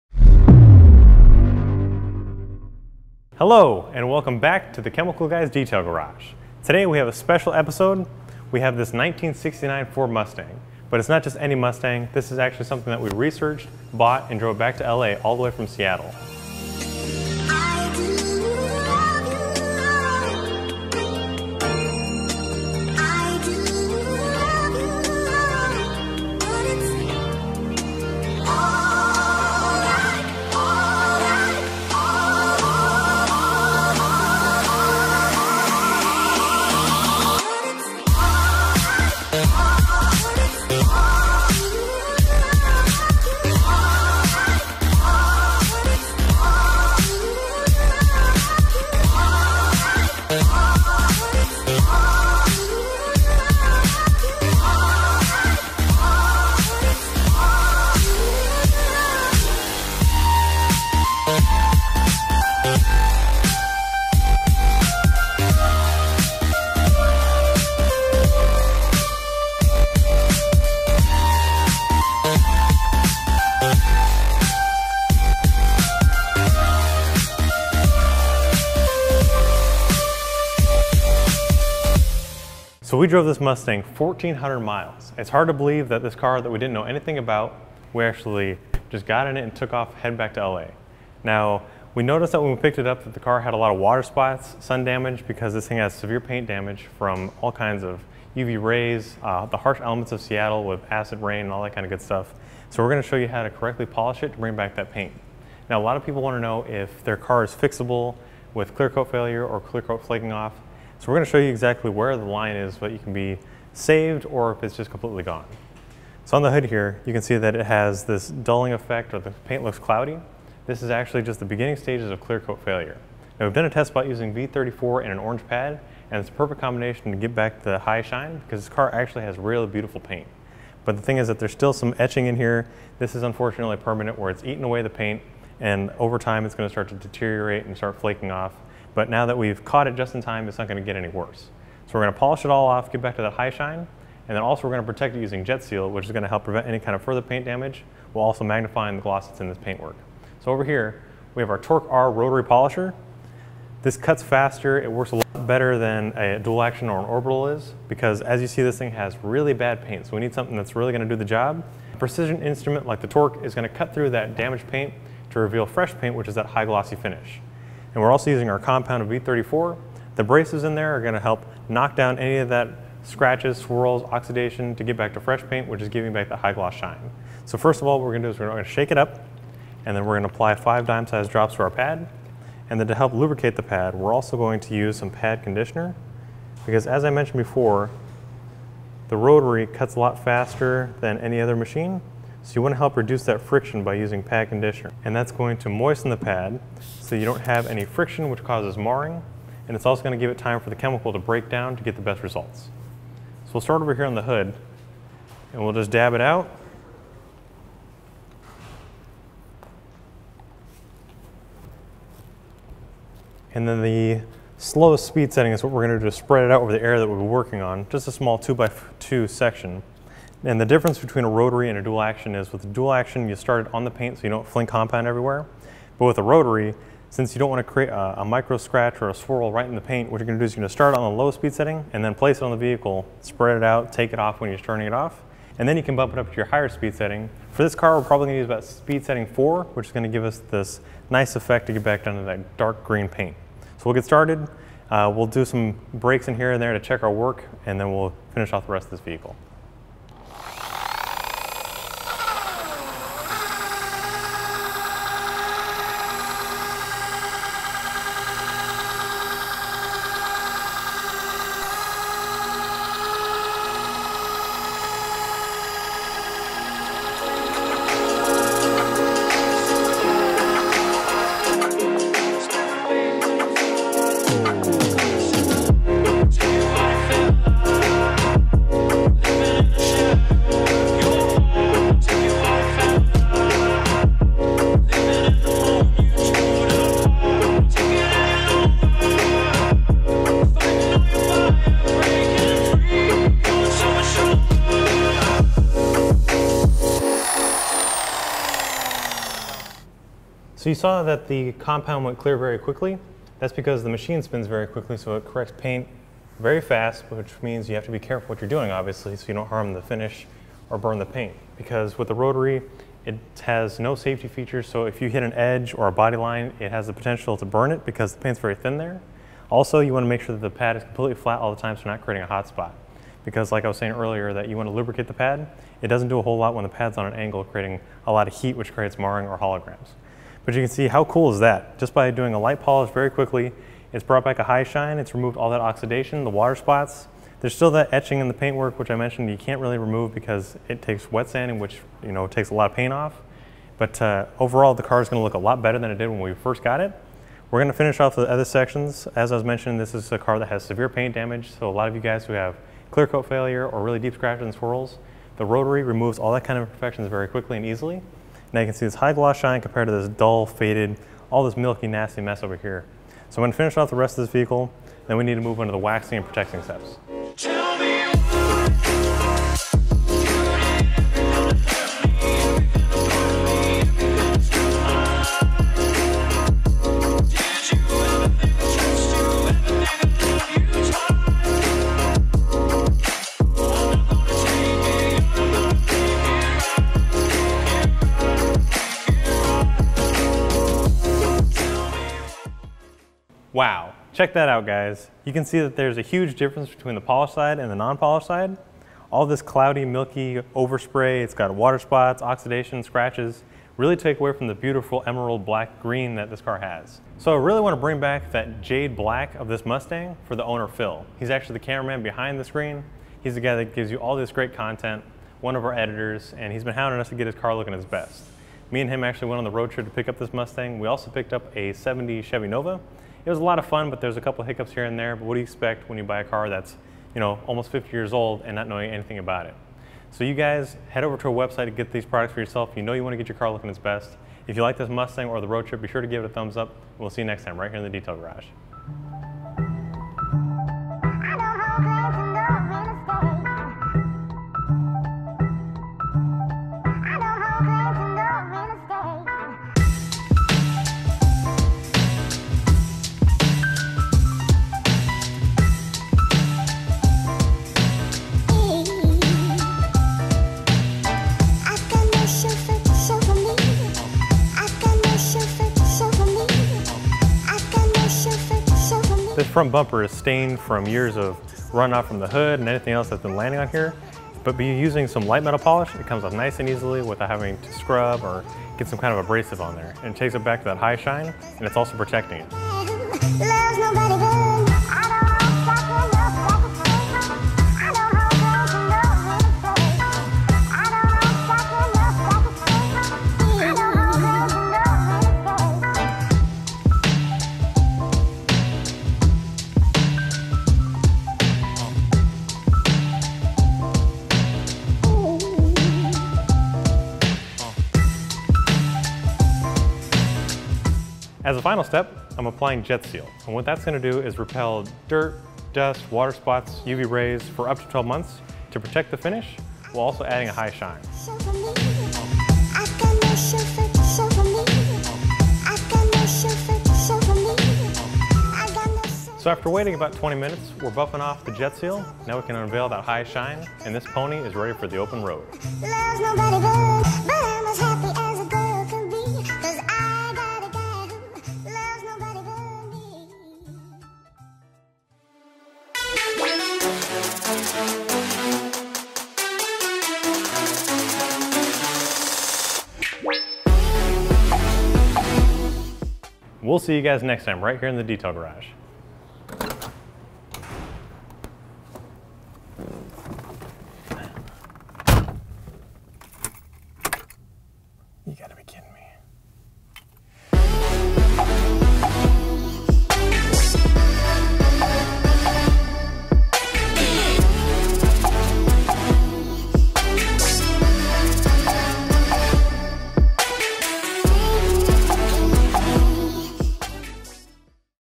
How To DIY Repair REALLY Bad Paint? | 1969 Mustang | Chemical Guys Car Care
In today's episode, you will see the journey that some of our media guys went on to bring this 1969 Mustang back to life!

They drove over 1,400 miles through the mountains, hills of Seattle, plains of Oregon and desert back to Los Angeles, and got the car filthy inside and out with dirt, bugs, brake dust, fast food, spilled drinks,and sweat.

In the comfort of the Detail Garage we can now thoroughly inspect the paint of this classic pony car. After washing and decontamination using a clay bar the paint still shows signs of poor washing habits and years of oxidation which will require polish to restore.

Machine polishing is known as the “art of detailing.” A skilled detailer can restore paintwork by removing scratches, swirl marks, and defects from clear coat paintwork. Using a machine polisher takes many hours of training and years of practice to master the art. In this video, Nick covers the full process on how to remove swirl marks and scratches from  paintwork using a rotary polisher to remove the defects, and restore the shine. Enjoy the video!